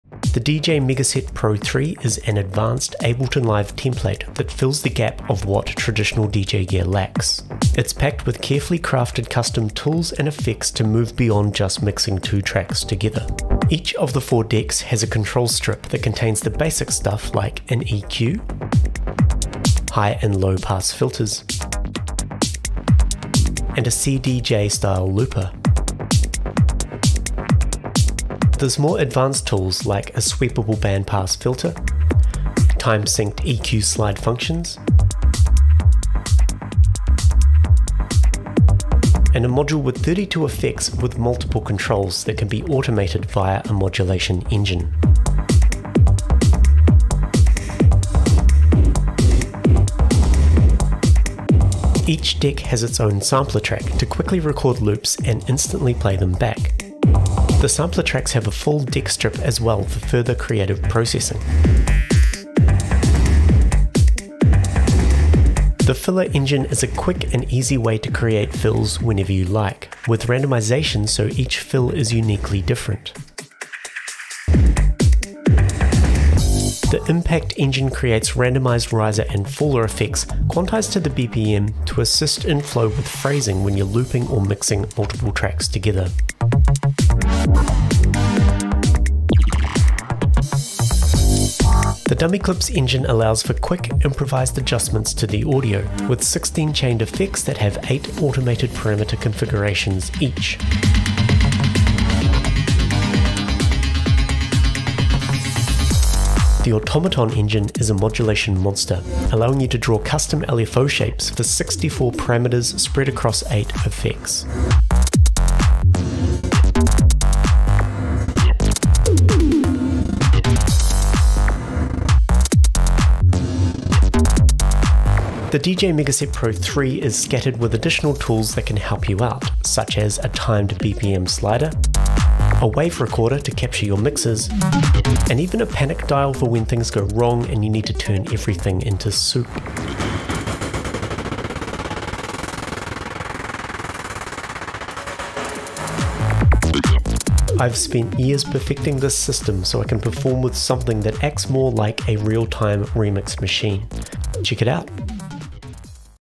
DJ Megaset Pro 3 - Advanced Ableton Live Template
The DJ Megaset Pro 3 is an advanced Ableton Live Template that fills the gap of what traditional DJ gear lacks.

It’s packed with carefully crafted custom tools and effects, to move beyond just mixing two tracks together

Each of the four Decks has a control strip that contains the basic stuff like an 3 band EQ, high and low pass filters, and a CDJ style looper.

The Deck Strip’s contain more advanced tools like a sweepable band pass filter, time synced EQ slide functions, and a module with 32 effects with multiple controls, that can be automated via a modulation engine.

Each Deck has its own sampler track, to quickly record loops and instantly play them back. The sampler tracks have a full Deck Strip as well for further creative processing.

The filler engine is a quick and easy way to create fills whenever you like, with randomization so each fill is uniquely different.

The Impact Engine creates randomized riser and faller effects, quantized to the BPM, to assist in flow and phrasing when you’re looping or mixing multiple tracks together

The Dummy Clips Engine allows for quick improvised adjustments to the audio, with 16 chained effects that have 8 automated parameter configurations each.

The Automoton Engine is a modulation monster, allowing you to draw custom LFO shapes for 64 parameters spread across 8 effects.

The DJ Megaset Pro 3 is scattered with additional tools that can help you out, such as a timed BPM slider, a WAV recorder to capture your mixes, and even a PANIC dial for when things go wrong and you need to turn everything into soup.

I’ve spent years perfecting this system so I can perform with something that acts more like a real-time remix machine. If you want that extra element of creative control when mixing music together, go check it out.

SOFTWARE REQUIREMENTS

Ableton Live Suite 11.x

SUGGESTED MINIMUM SYSTEM REQUIREMENTS

Included in the ZIP file is a “Light” version that has stripped back functionality if you find the template can not run on your system.